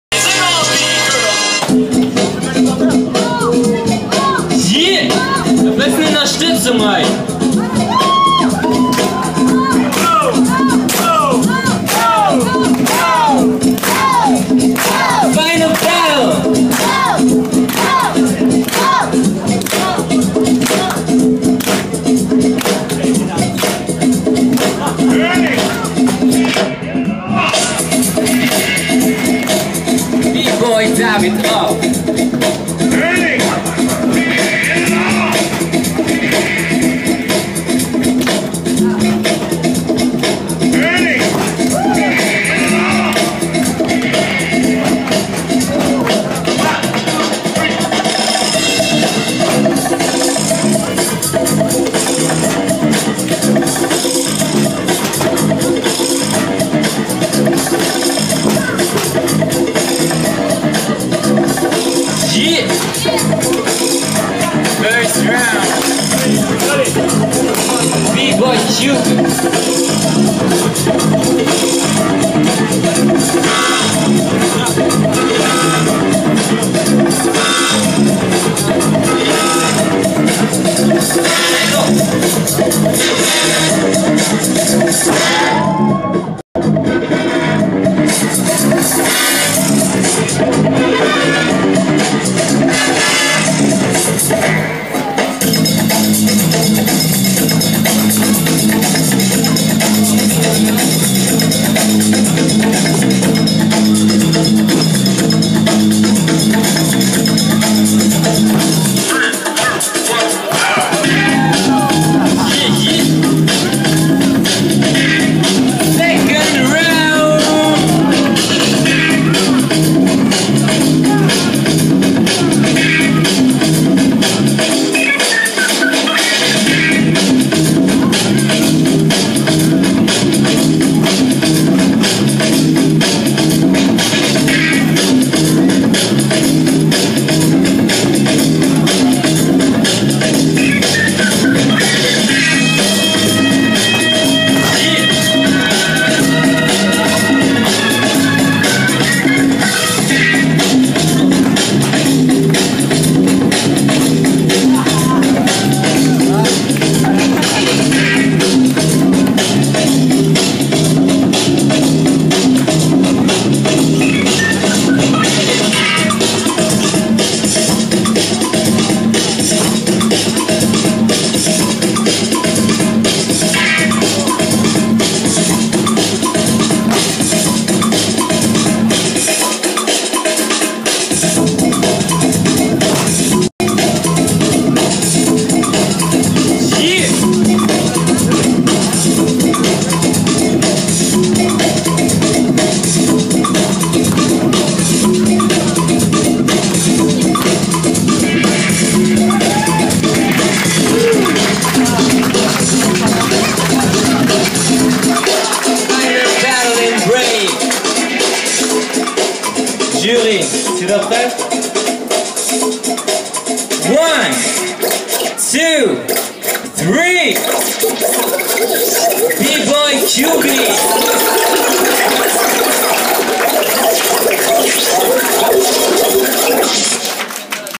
GOOD VIBES II Battle - Break Final Bboy Kiubi vs. Bboy David-off
Winner: BBoy Kiubi (left)
Congratulations

Good Vibes II Battle @ Urban Art Festival - Kufa / Esch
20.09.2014